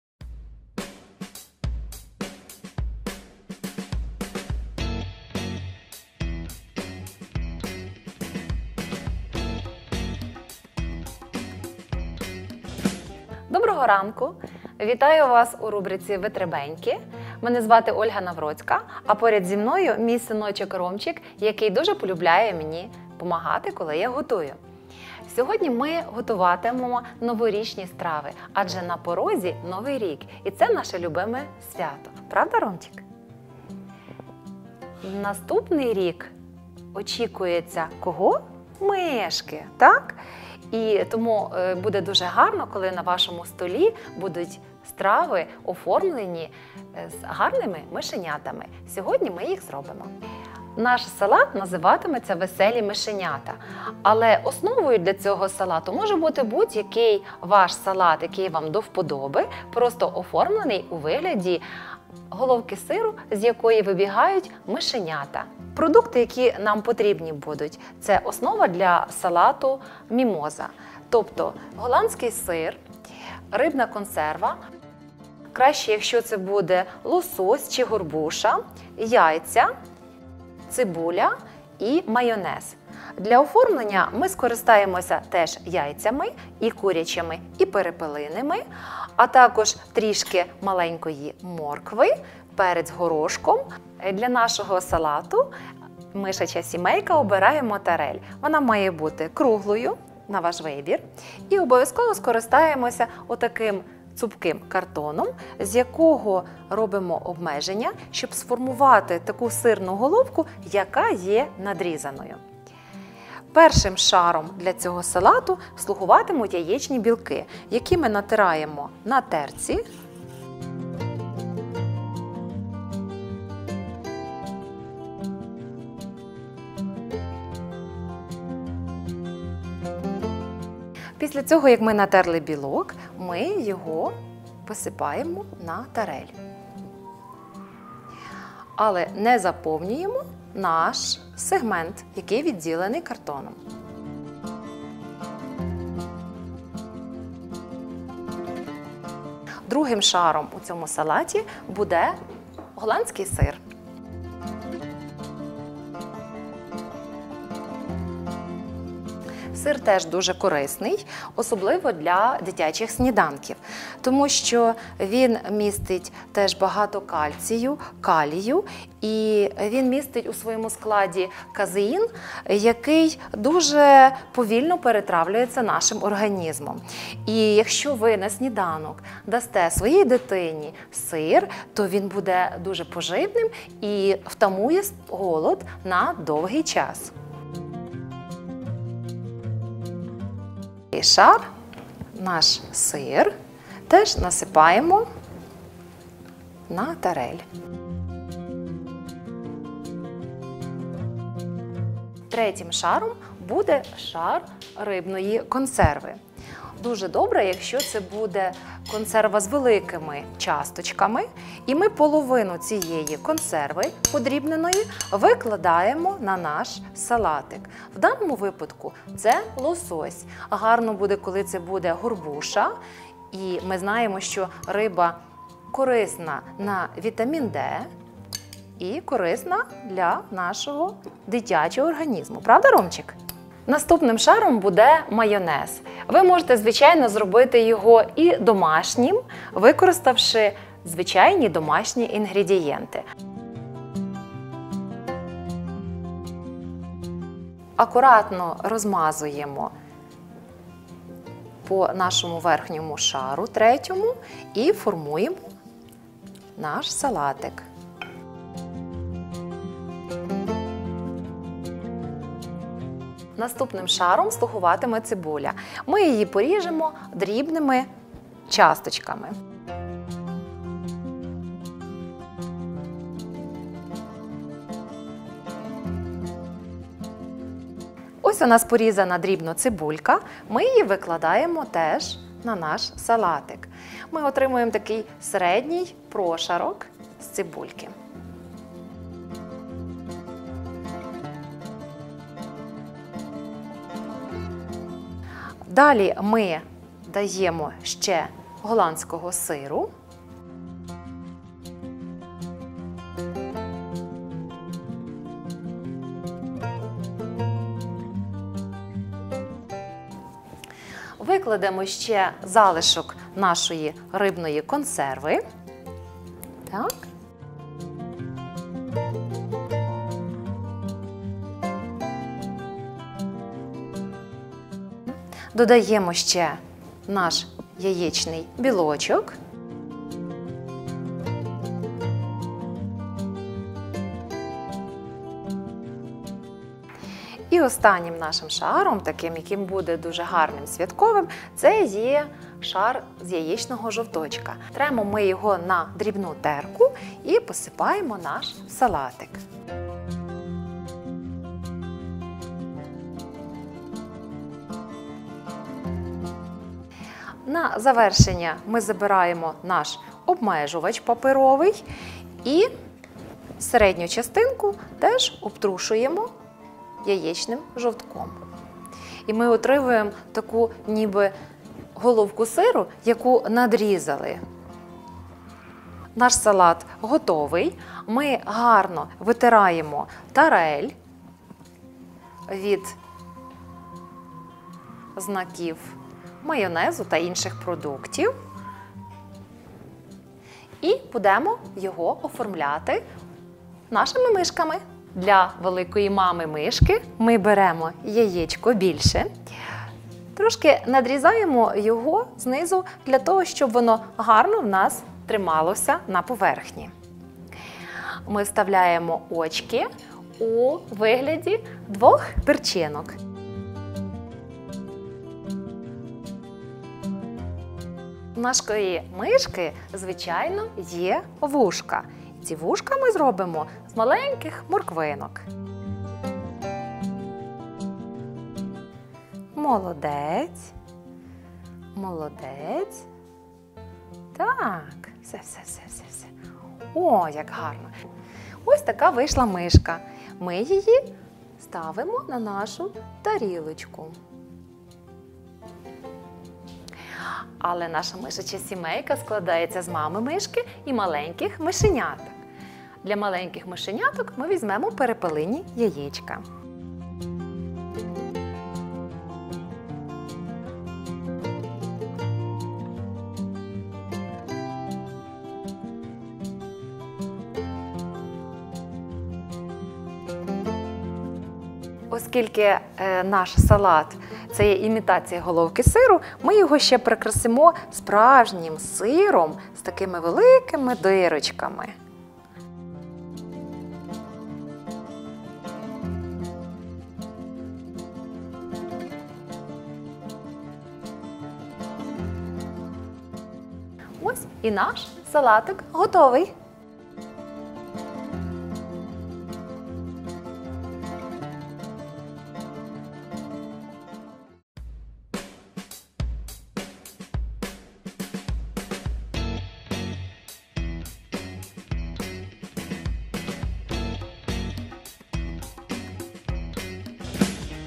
Рубрика "Витребеньки" - Новорічна страва
А ви вже знаєте, яку оригінальну страву приготуєте до новорічного столу?  У Рубриці "Витребеньки" разом з Ольгою Навроцькою покажемо вам як приготувати особливий салат для вашої вечірки!